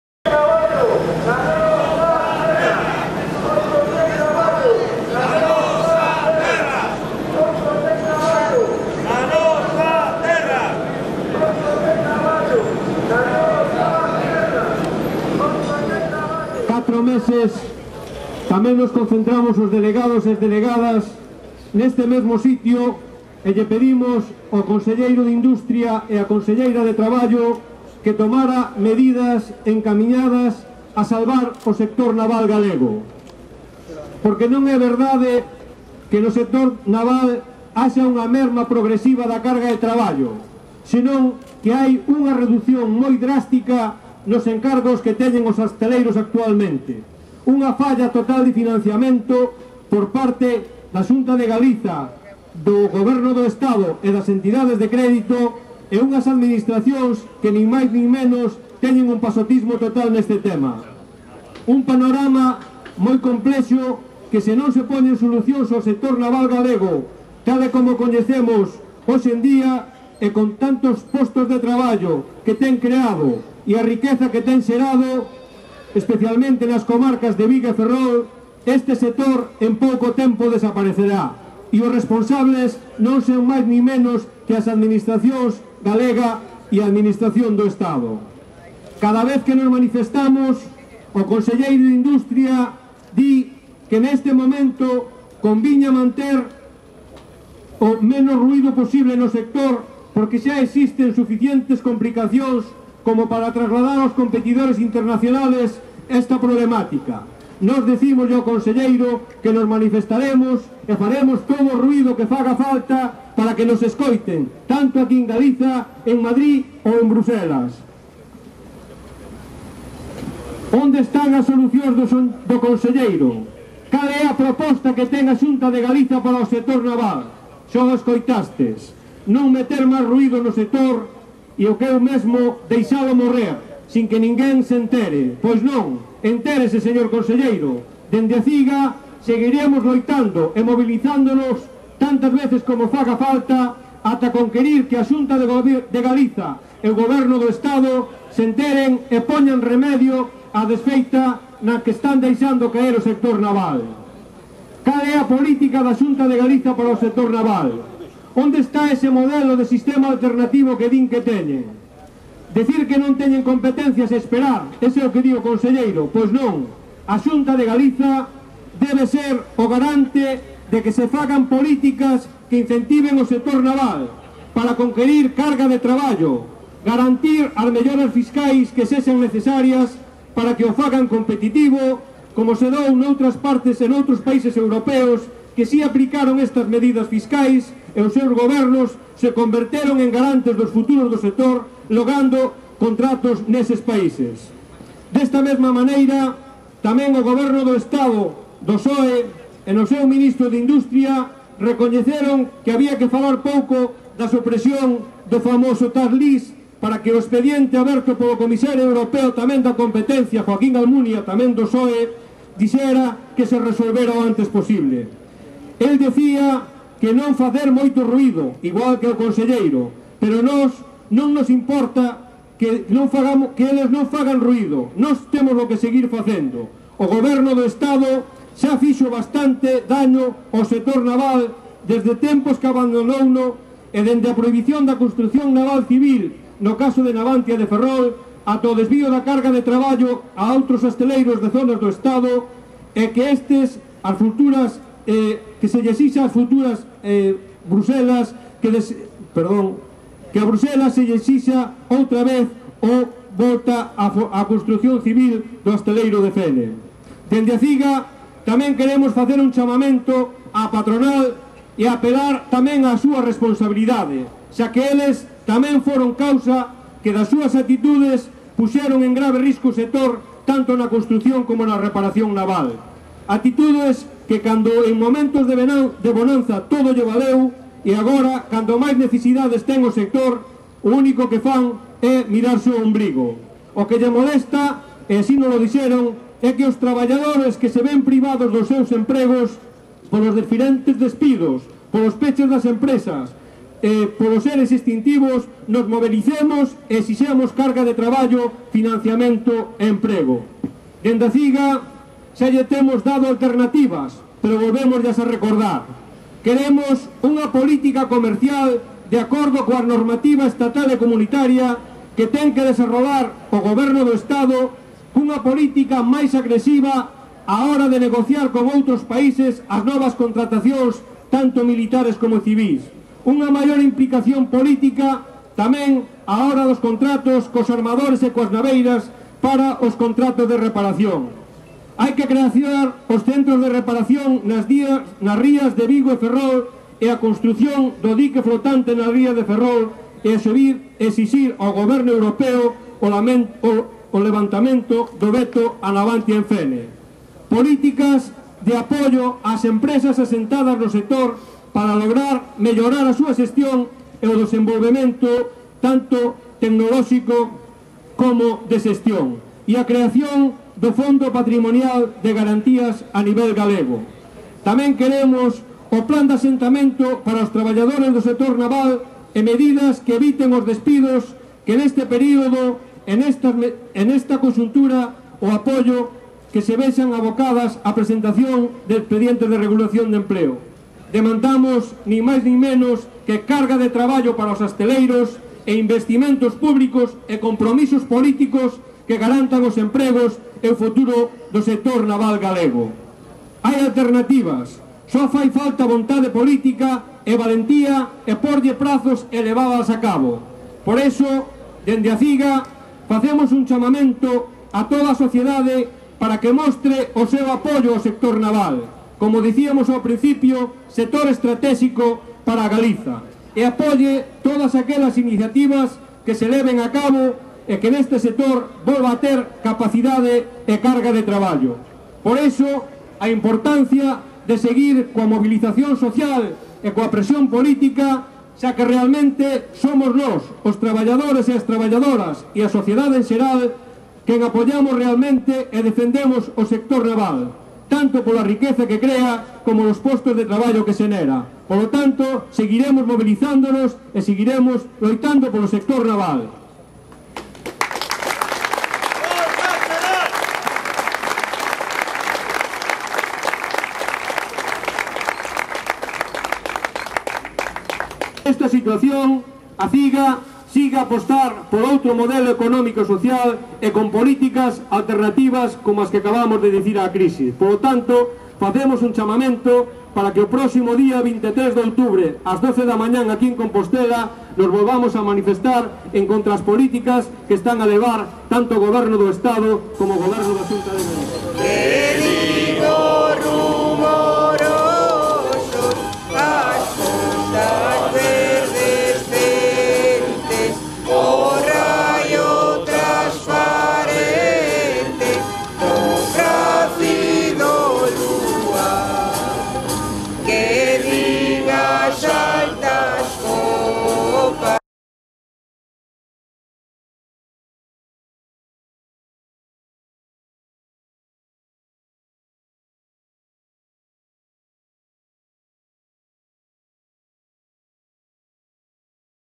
Manifestación CIG-Metal en defensa do naval (Compostela 4/10/2011).mp4
Manifestación de delegados de CIG-Metal en defensa do sector naval galego o 4 de outubro do 2011, en Compostela. Intervención de Xoán Xosé Bouzas, Secretario Nacional de CIG-Metal.